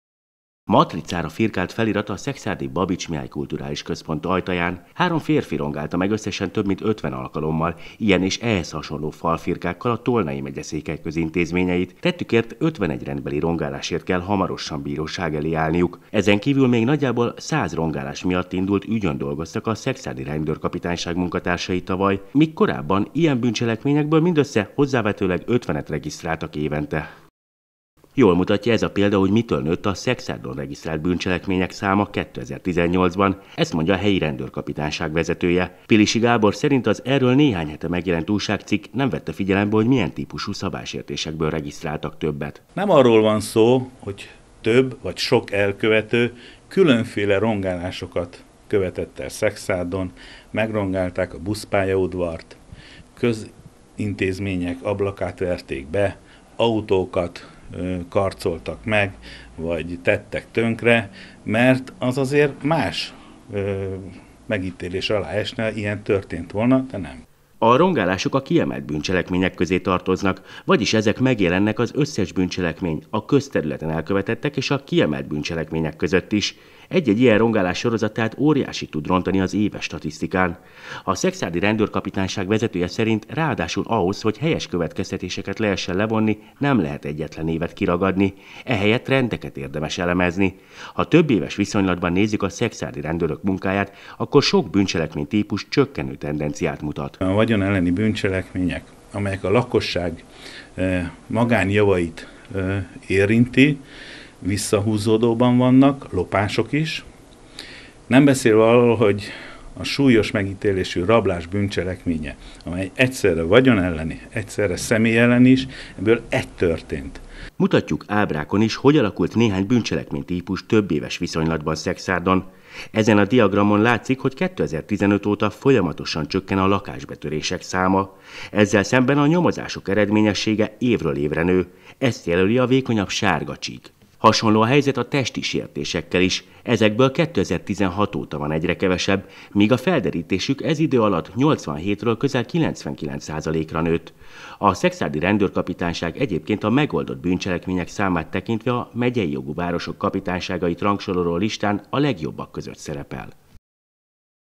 Cáfolat - a bűn városa Szekszárd 20190801
A bűn városaként írt néhány hete Szekszárdról egy újságcikk. Az írás azt állította, hogy a tolnai megyeszékhelyen volt tavaly a legtöbb lezárt nyomozás. Ezt a helyi kapitány sem vitatja. Pilisi Gábor viszont azt mondja, egyáltalán nem mindegy, hogy az emelkedés falfirkák, vagy például súlyos testi sértések miatt következett-e be. Előbbiből egyébként tavaly nagyjából 150-et regisztráltak tavaly Szekszárdon.